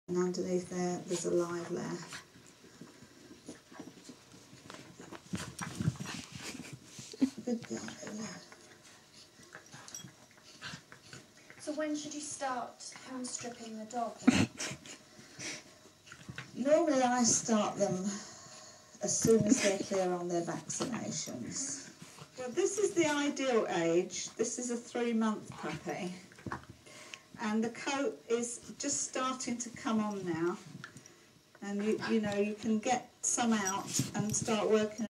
Schnauzer Play
Freddy and a friend's Mini playing about like mad. Ignore the Schnauzer grooming video in the background and the weird snorting noise I make.